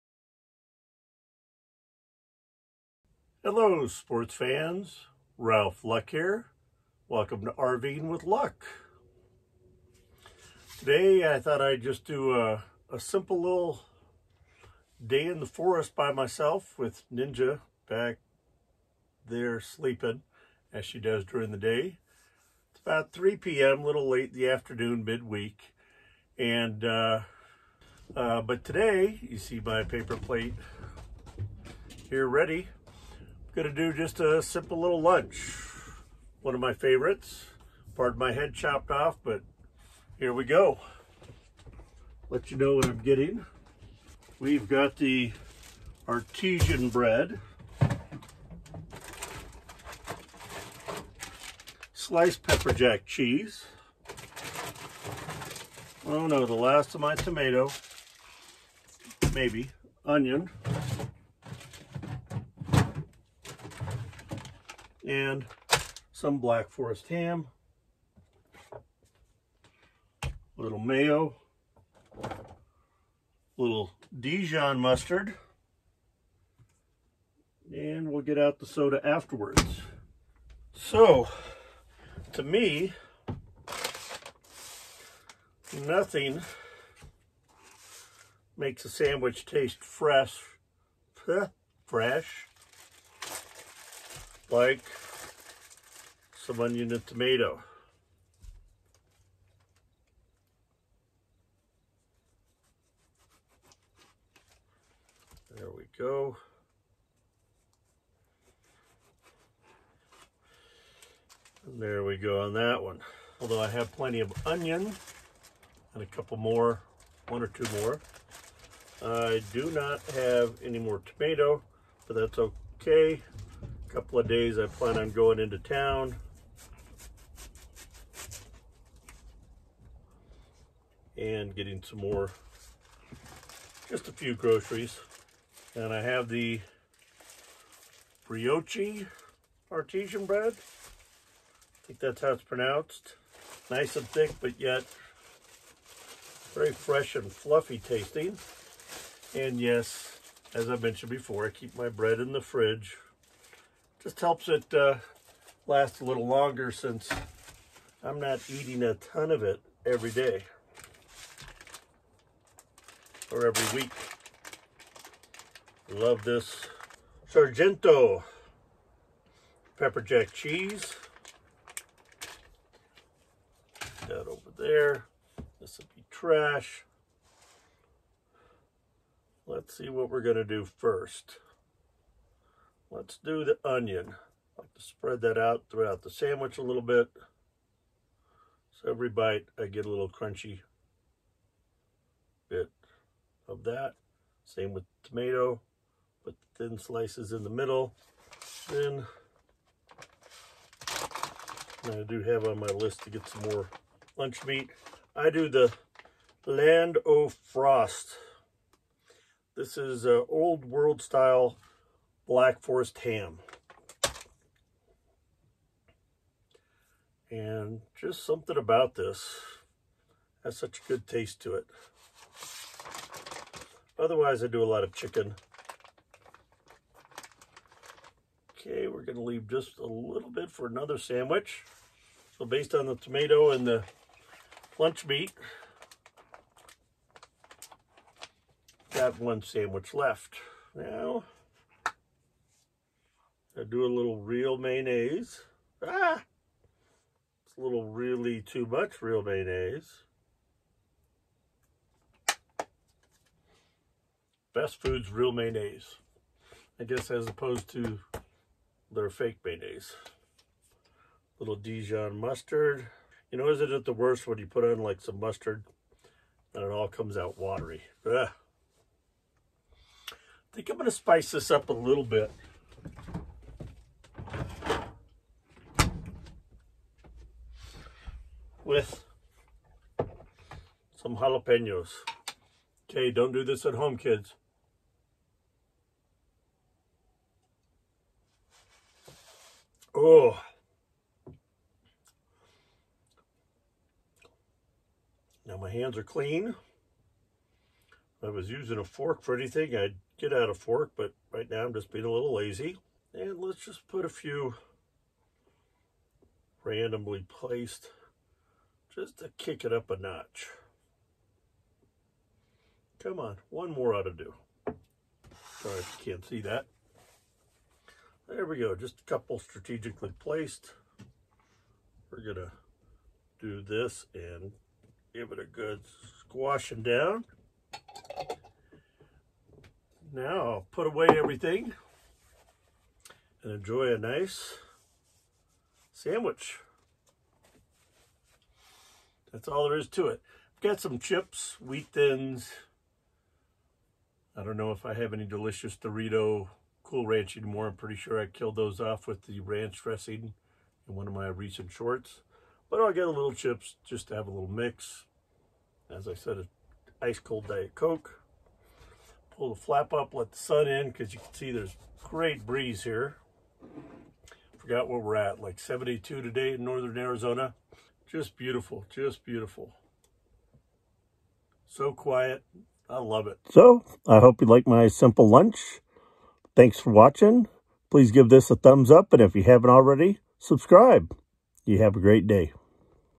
RV lunch//Full-time RV life
Nothing fancy, just a simple sandwich. But added sliced jalapeños, tomatoes and onion.

Be positive.
Be nice.
Be active.